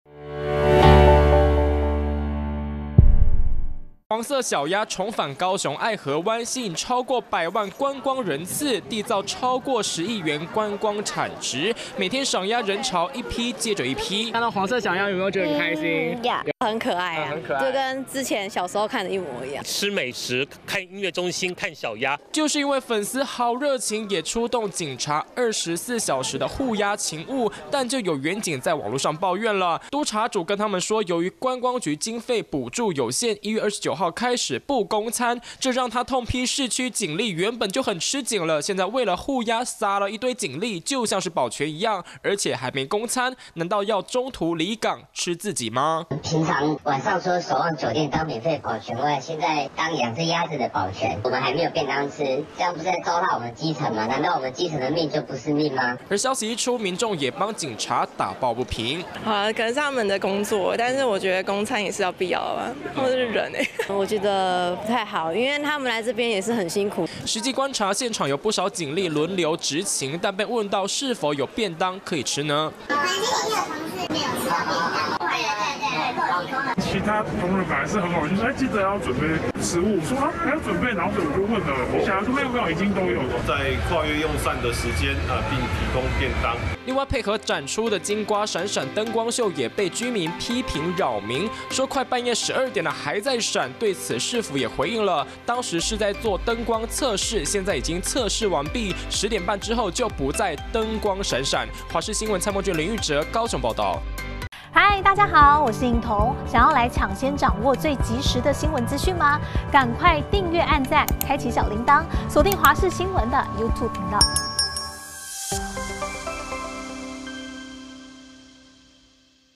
警顧黃小鴨爆「沒便當吃」 警局：行動補給站供餐｜華視新聞 20240202
黃色小鴨睽違10年重返高雄愛河灣，根據觀光局統計，展出一週就已經吸引破百萬人次賞鴨，更締造10億元以上觀光商機，不過就是因為人潮太多，員警也疲於奔命，近期有基層員警抱怨，為了24小時的「護鴨勤務」居然因為高層的一句「經費有限」，就沒有供應便當了，不過對此，高雄市警局也回應了，表示在展區周邊都有設置行動補給站讓員警補充體力，如果遇到用餐時段，也會供餐。